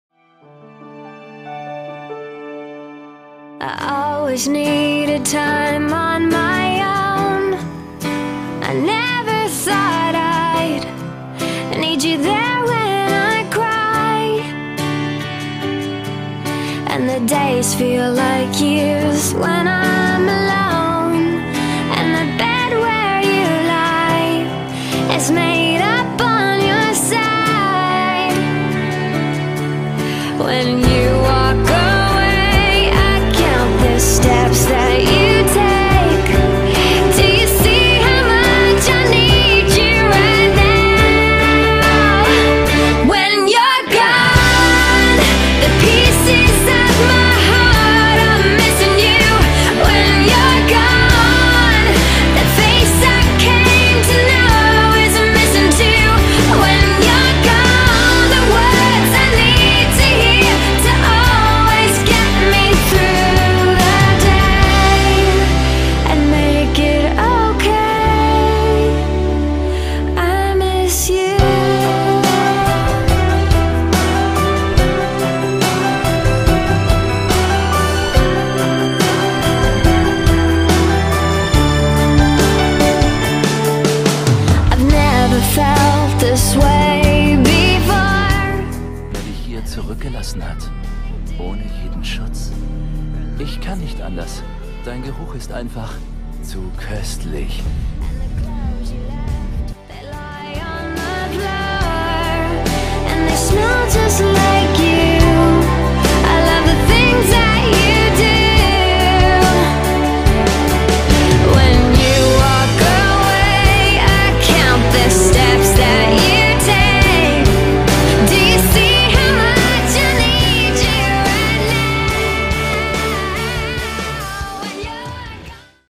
Scherbenmeer trailer
Das ist der Trailer zu meiner Wattpad Geschichte "Scherbenmeer".
Freu mich über Feedback.